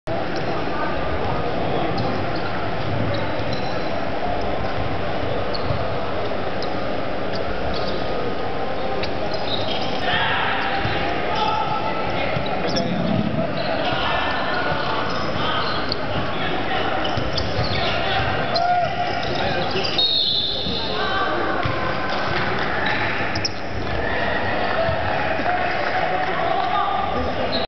G-Net vs R&P 13
August 20, 2006, at Basketball Association of Singapore
G-Net playing against R&P